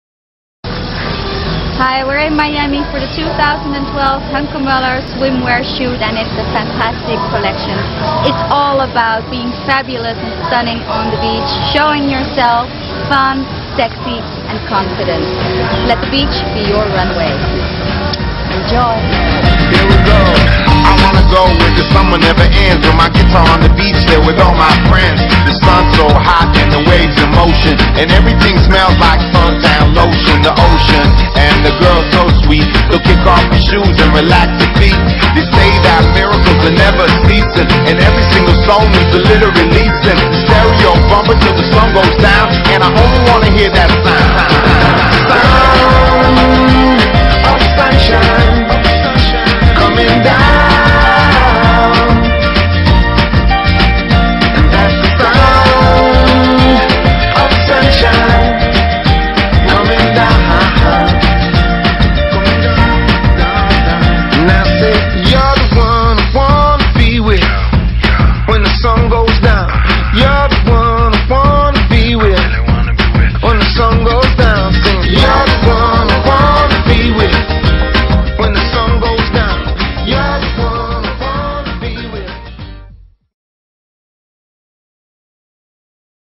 Behind the Scene Hunkemöller Sylvie van der Vaart - Meis
Behind the Scene van de Hunkemöller commercial met Sylvie van der Vaart - Meis